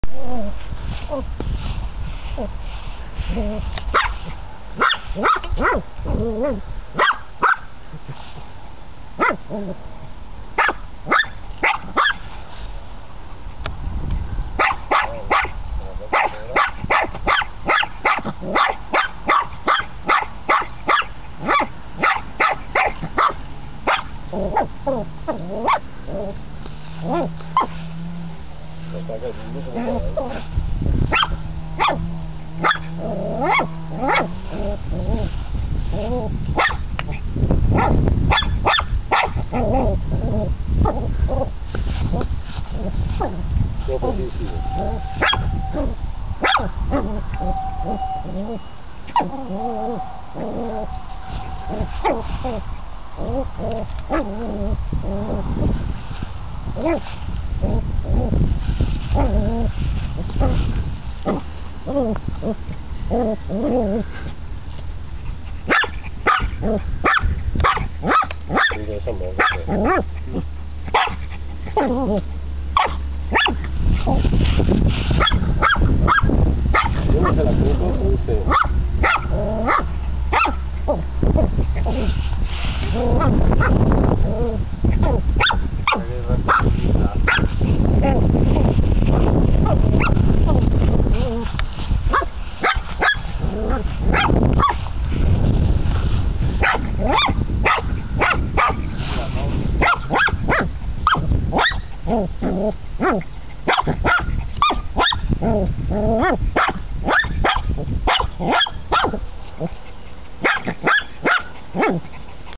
Volpina vs Talpa
La mia volpina Sissi contro una talpa.